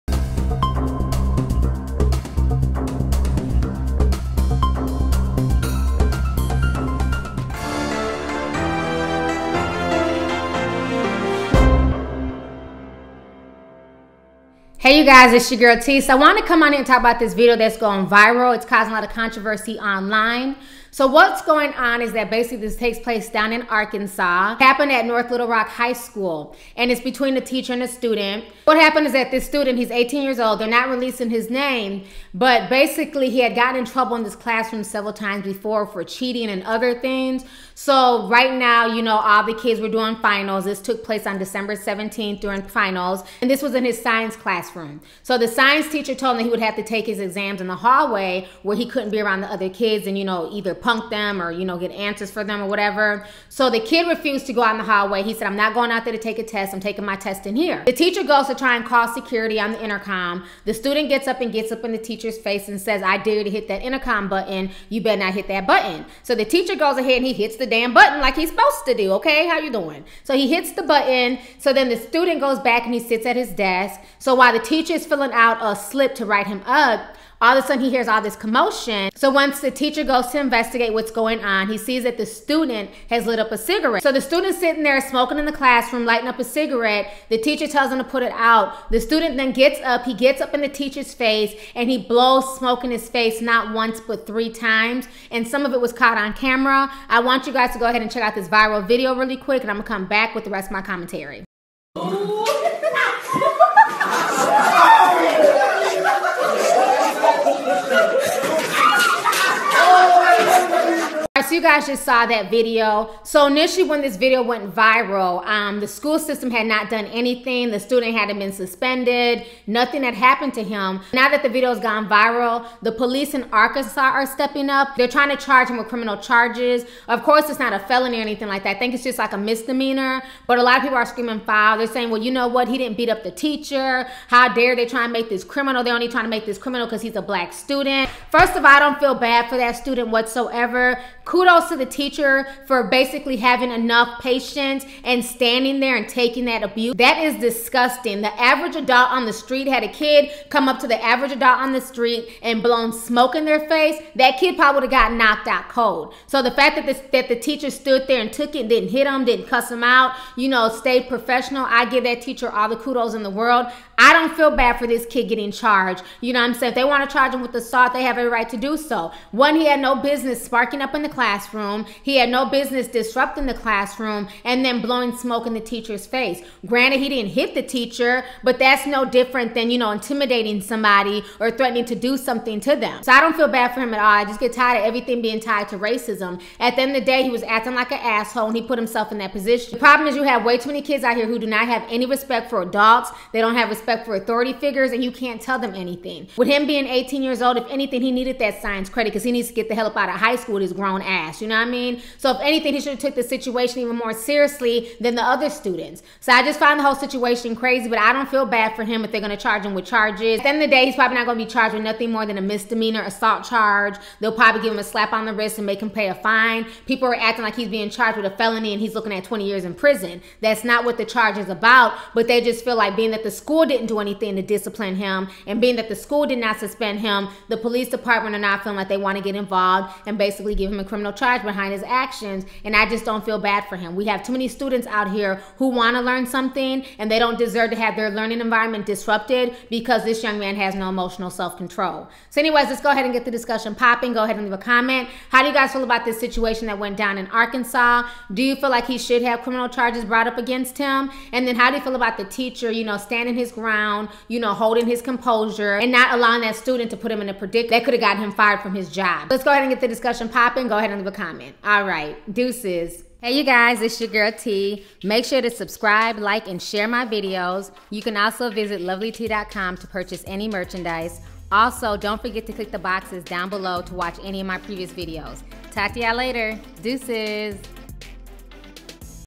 Arkansas Student Who Blew Cigar Smoke In Teacher’s Face Now facing Criminal Charges
Arkansas Student Who Blew Cigar Smoke In Teacher’s Face Now facing Criminal Charges  this is my opinion of the situation.